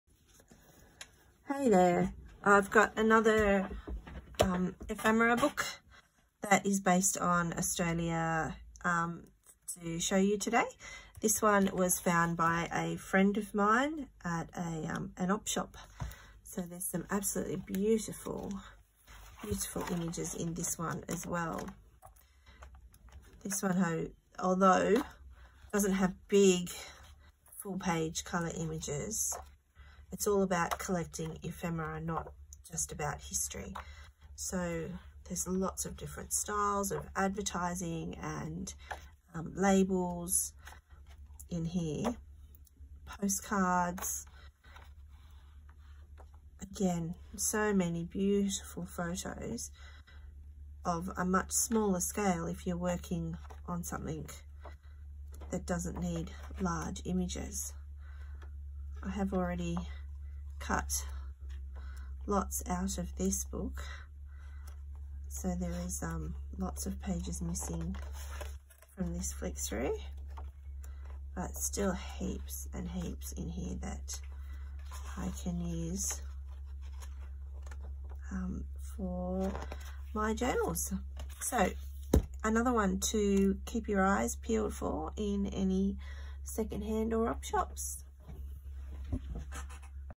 1 October 2020
Book flip through of vintage ephemera.

Carter's
Yesterday's Paper-  Collecting Ephemera in Australia
Vivienne O'Neill
John Furphy Pty. Ltd
Date Unknown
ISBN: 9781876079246